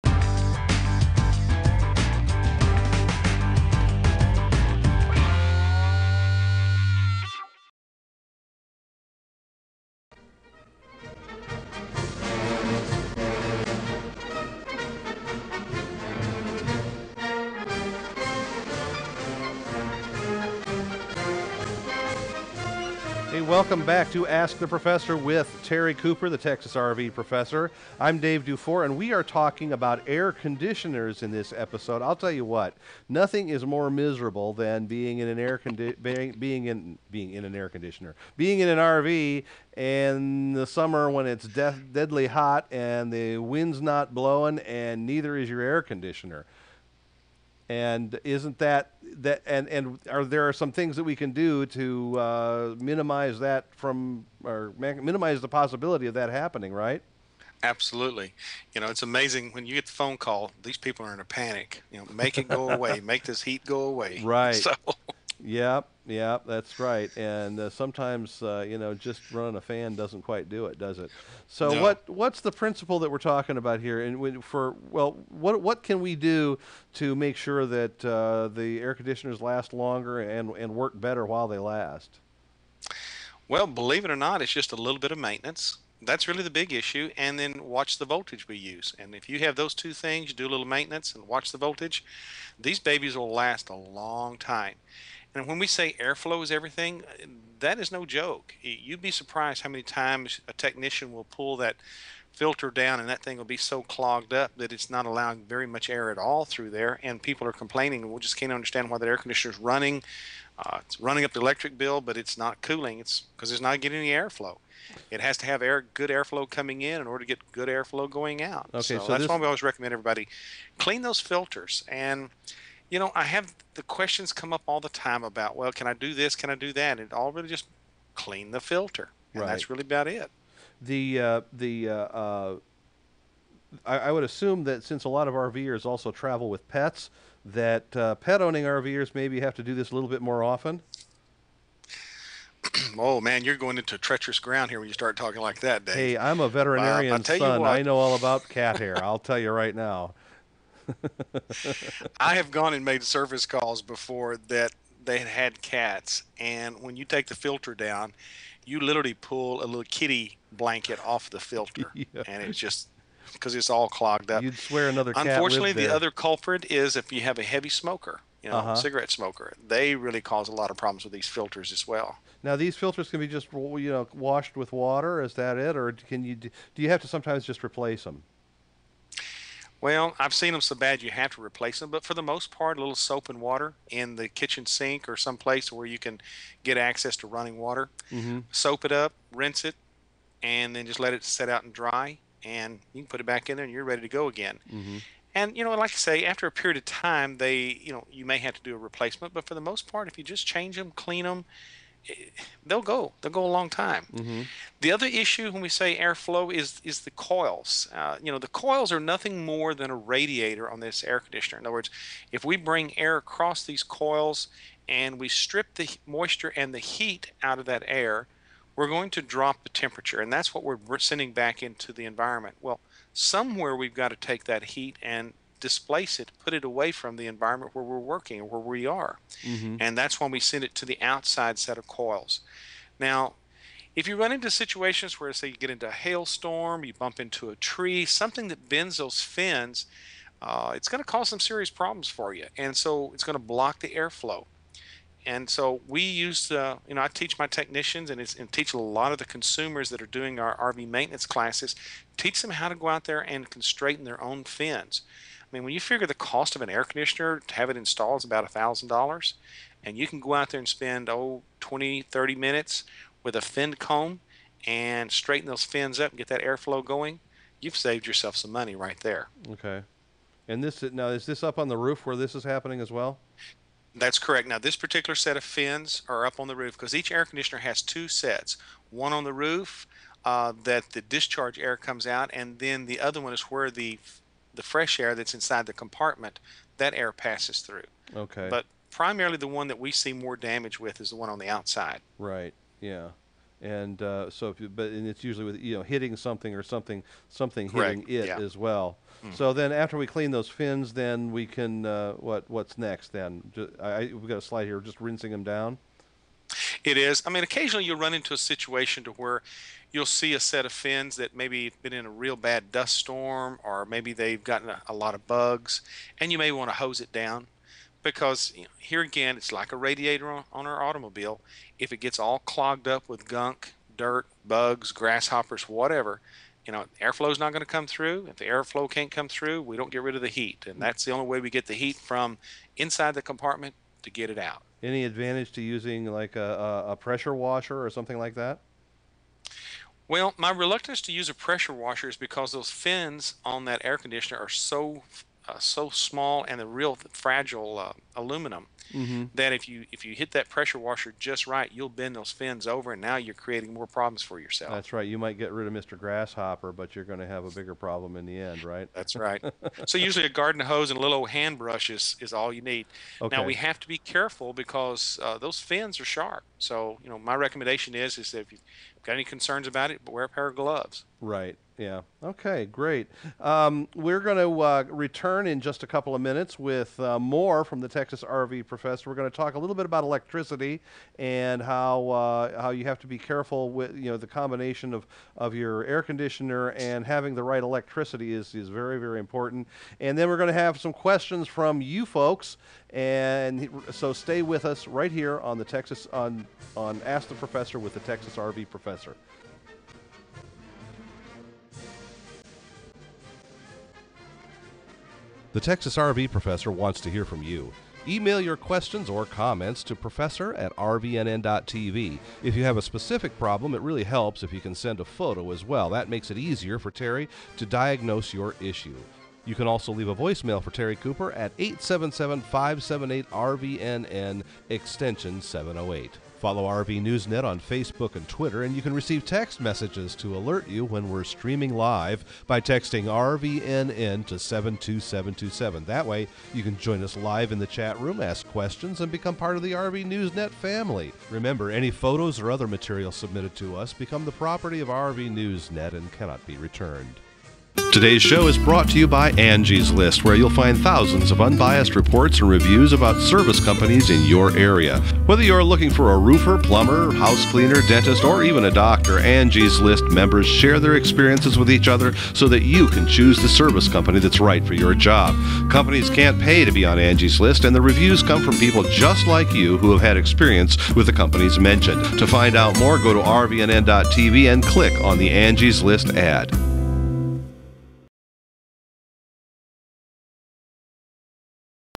Ask the Professor: A/C in your RV
Air Conditioning in your RV

It's not going to get cold, there's just going to be less heat. A/C is actually stripping the heat out of your RV, controlling the humidity, transferring heat, controlling temperature, and ventilation. A little maintenance and watching your voltage will give your a/c a long life!

Slide Bullets:
Your RV Air Conditioner
BTU
Air Flow is Everything
Cleaning the Air Filter (HowTo)
Bad Volatge-The RV Killer (Good/Bad #'s)

Viewer questions answered by the Professor.

Want more great programming like this?

Facebook.com/RVNewsNet
text RVNN to 72727

You can find all your favorite RVNN programs on iTunes by searching for RVNN!